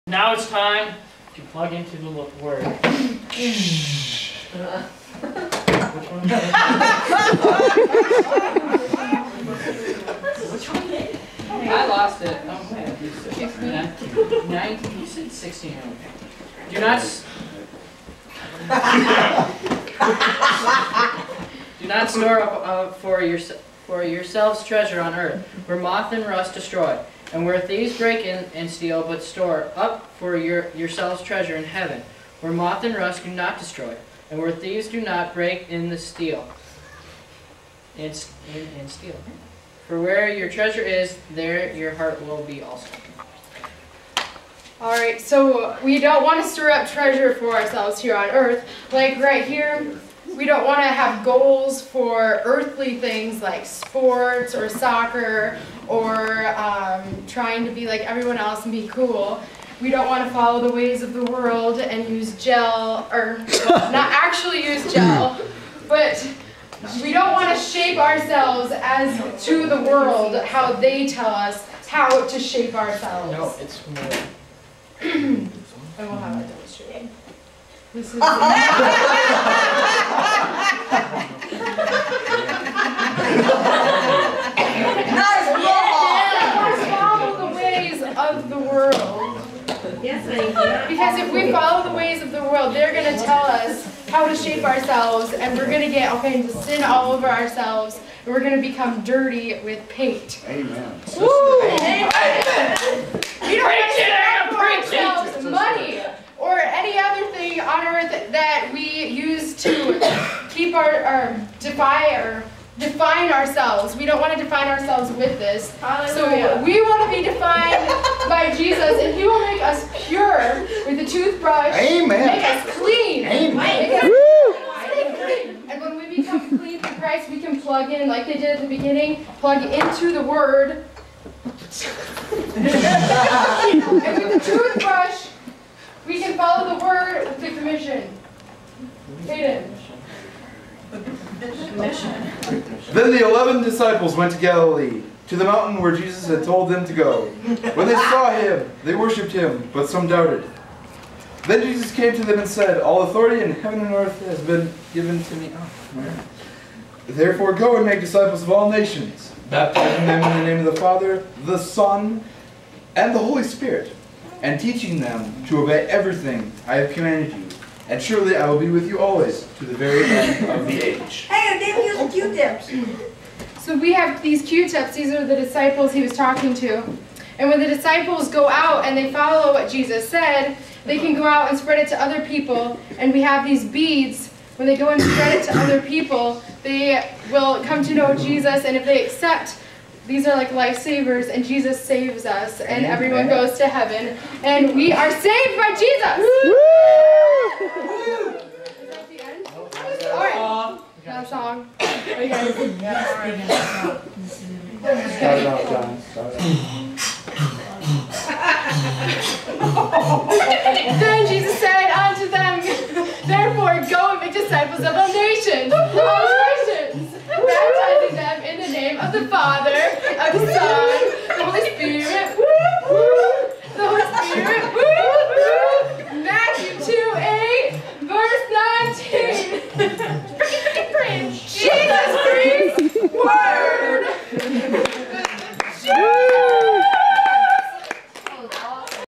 3 minute sermon(s) group 4
3 minutes to prepare a sermon here's how they did